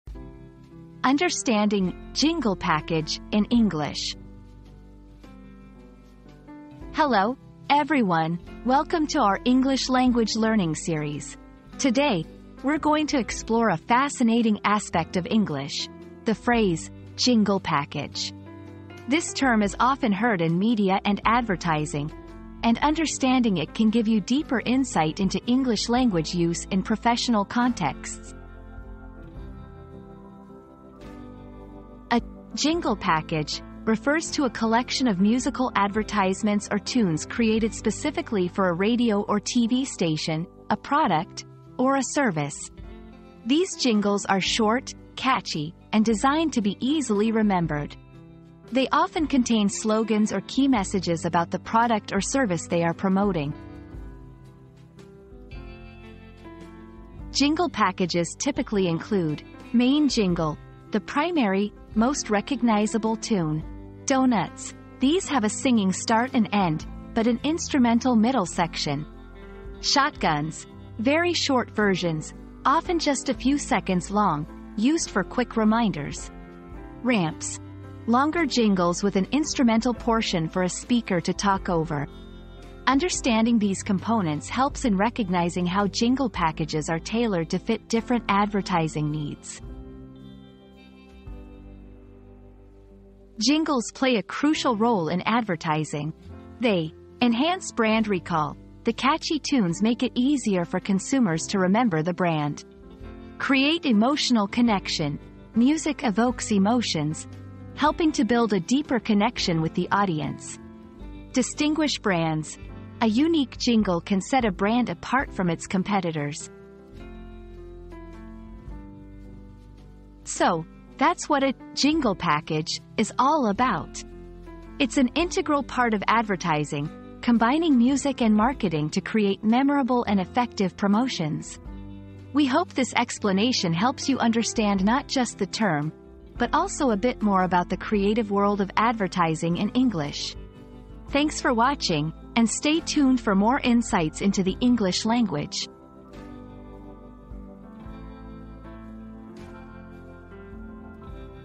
Understanding "Jingle Package" in English
Unraveling the Mystery of the 'Jingle Package' in English • Discover the meaning and significance of the intriguing 'Jingle Package' term in English. Join us as we delve into its origins, usage, and cultural impact, providing you with a fascinating insight into this unique linguistic phenomenon.

00:00 • Introduction - Understanding "Jingle Package" in English
00:31 • What is a "Jingle Package"?
00:56 • Components of a Jingle Package
01:32 • The Role of Jingles in Advertising